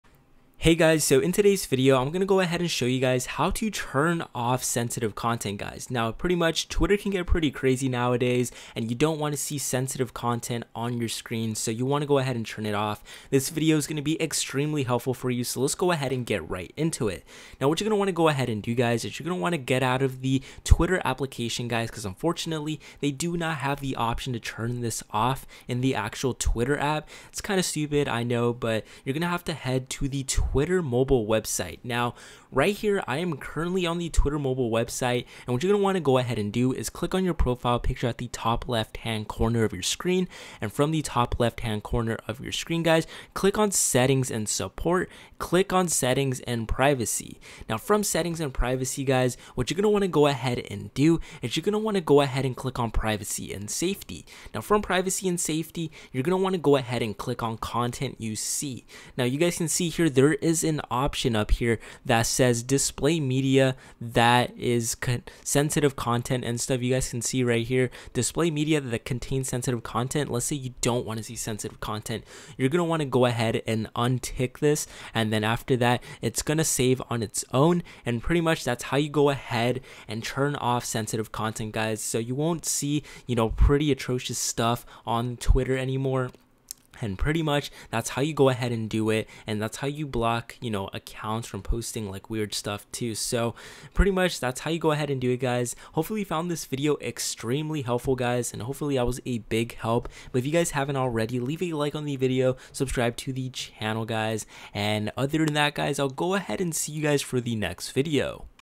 How to Turn Off Twitter Sensitive Content (2023)
Twitter is a great way to connect with your followers, but it can also be a dangerous platform if you're not careful.

In this video, I'll show you how to turn off Twitter's sensitive content setting, so that you can post content without fear of offending your followers. This setting is handy if you want to share personal information, images or videos that may offend some people.

If you're looking for ways to make your Twitter account more secure and private, then this video is for you! By learning how to turn off sensitive content on Twitter, you'll be sure to stay safe and protect your online privacy!